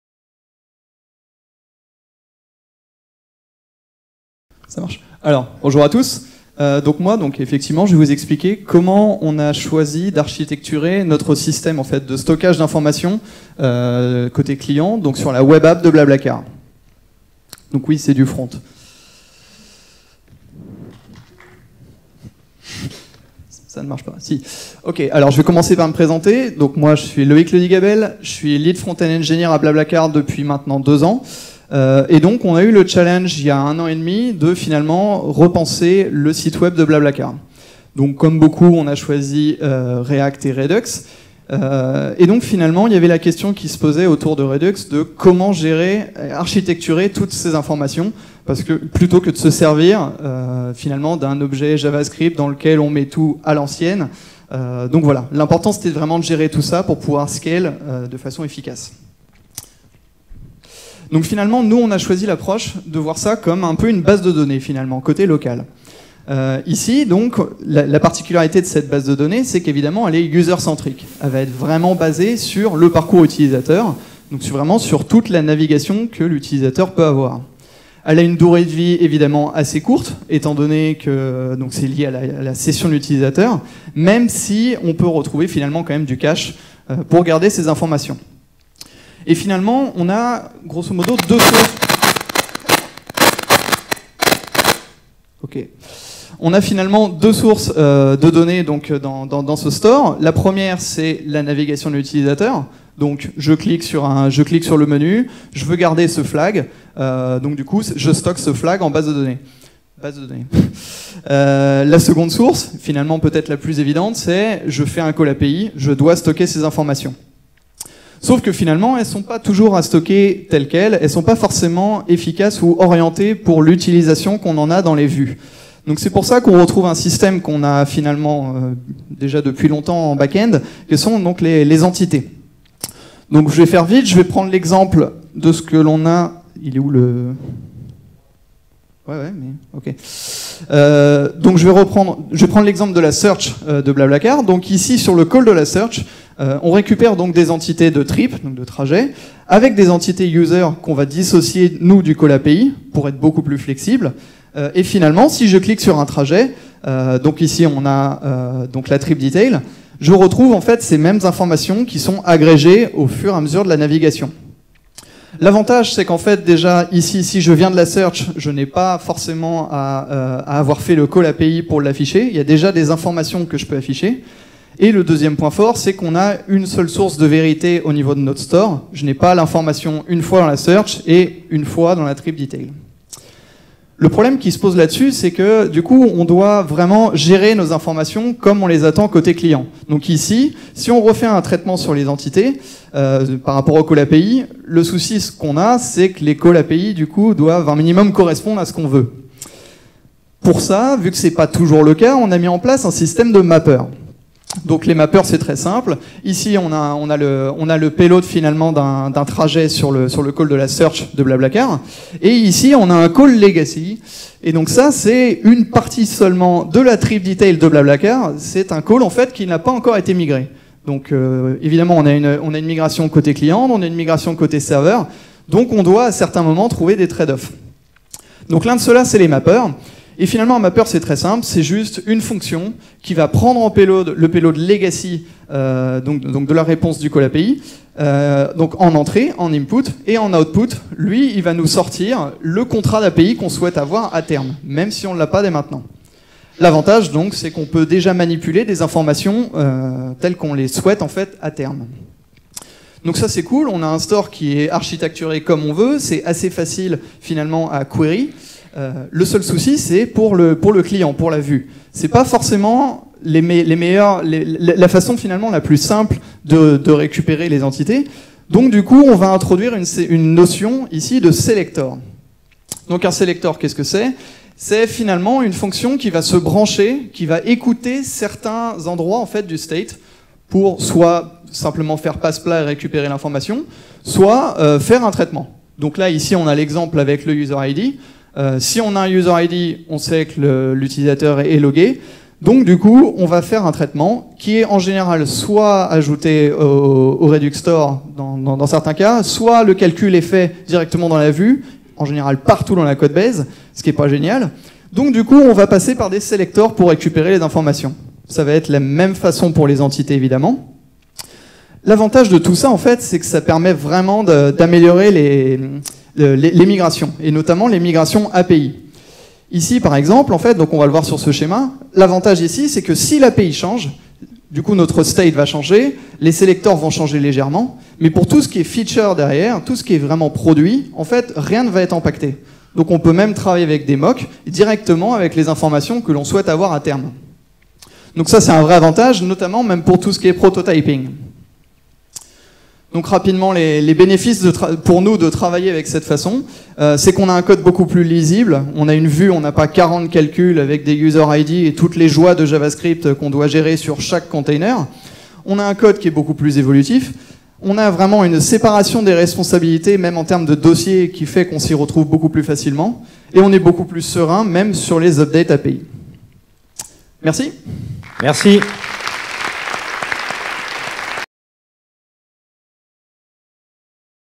How to architecture your client-side storage - Loïck Le Digabel
It is not always easy to deal with the client side informations aggregated across the navigation. Here is how we decided to deal with this problematic on the BlaBlaCar Single Page App.